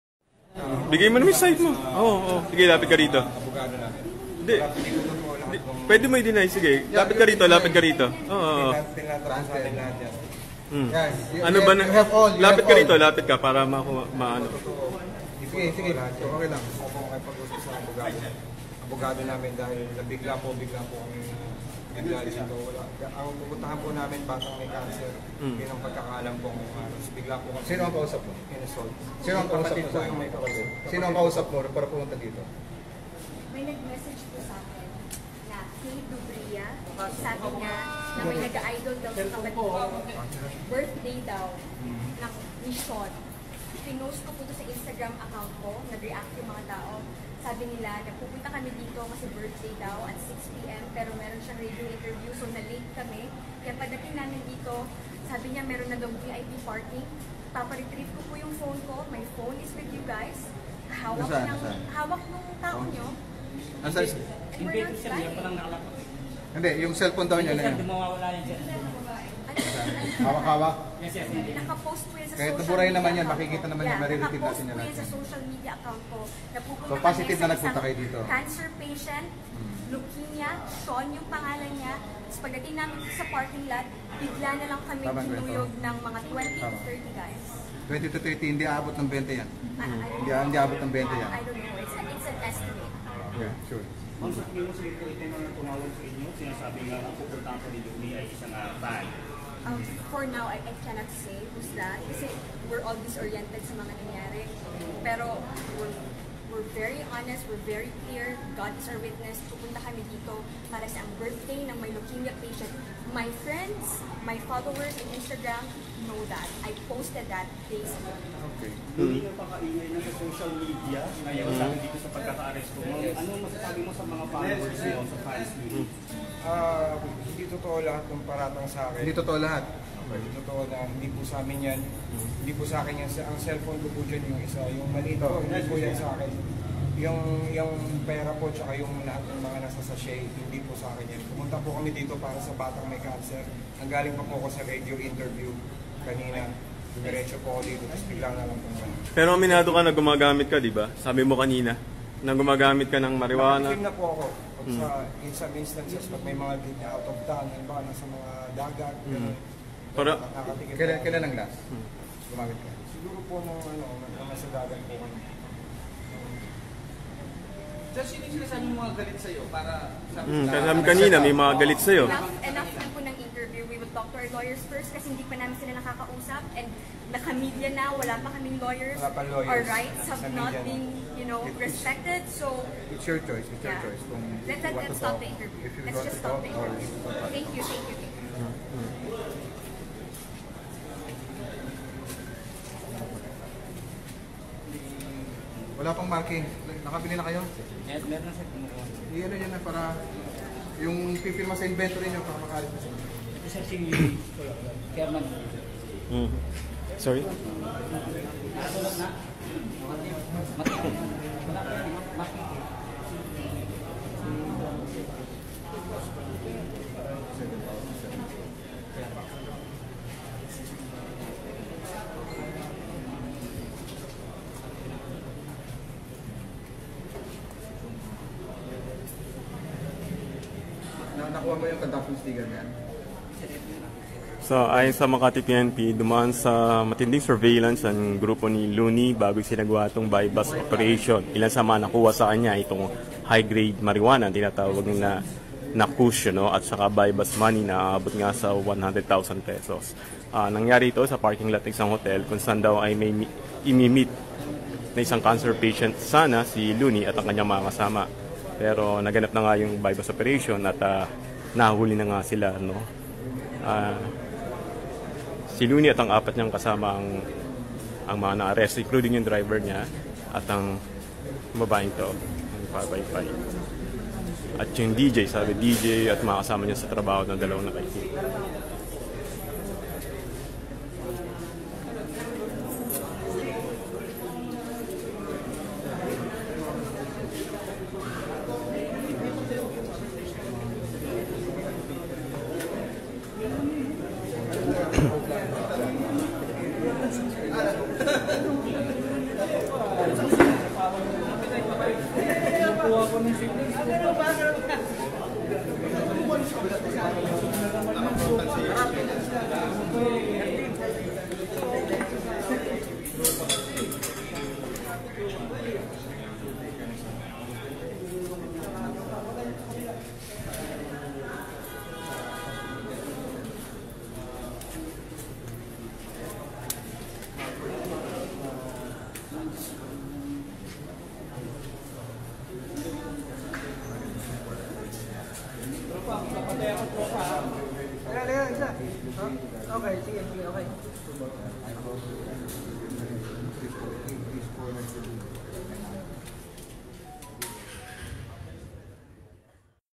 PANOORIN: RAPPER NA SI LOONIE ARESTADO SA BUY BUST OPERATION SA MAKATI
Metro: PANOORIN: JUST IN | Arestado ang Rapper na si Loonie at 4 na iba pa matapos ang isinagawang buy-bust drug operation ng mga pulis kagabi sa isang parking lot sa Brgy. Poblacion sa Makati City kagabi. RVG. NIAS News Philippines

Kanias I share ang post na ito para sa pinakasariwang balita at inpormasyon